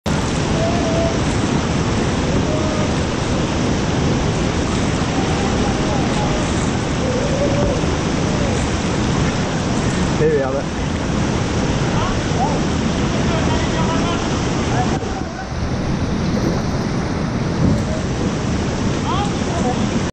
Ganière en kayak - Seuil du pont de Gournier - David
Descente de la Ganière (Ardèche) en kayak - plus d'infos - 27 avril 2019